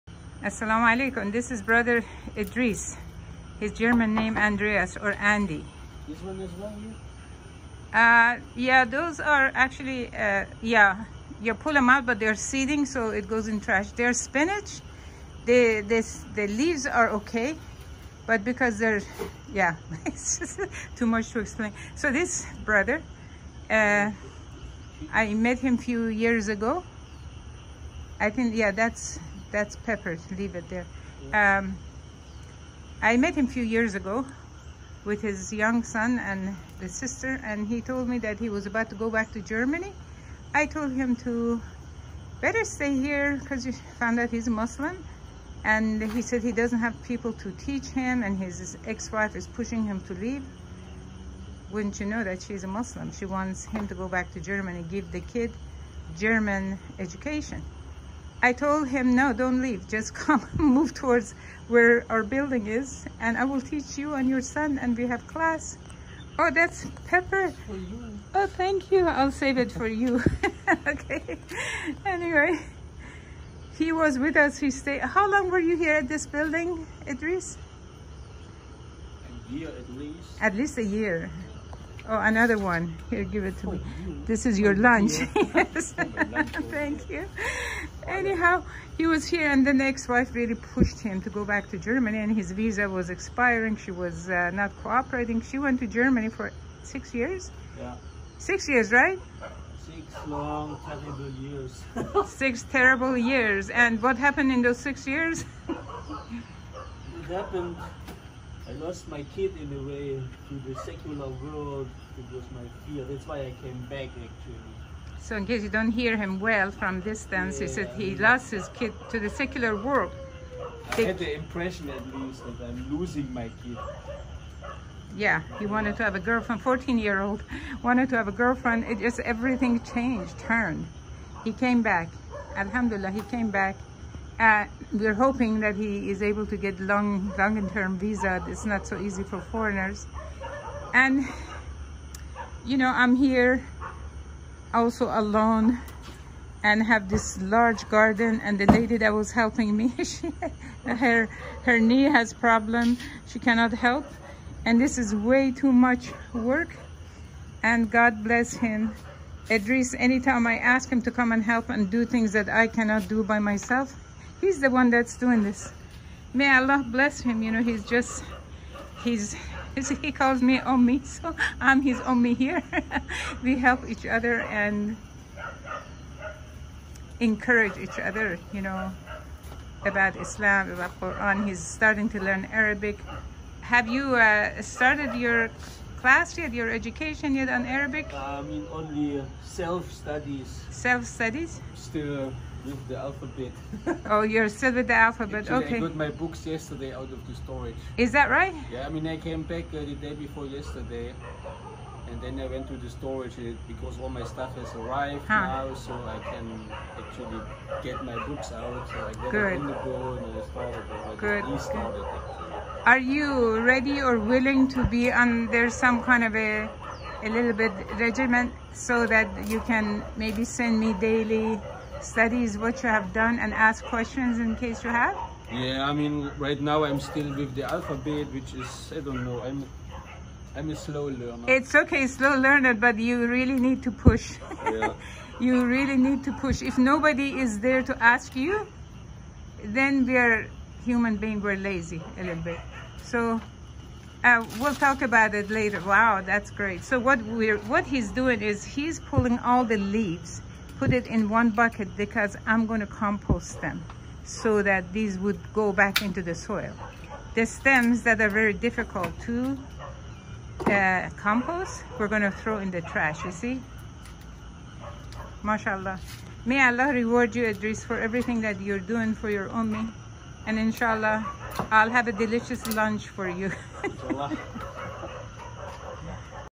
Wants to Learn Arabic! Miserable in Germany and happy in Malaysia. Great Garden Helper.
Why his Muslim ex-wife wanted him to go back to Germany with their kid?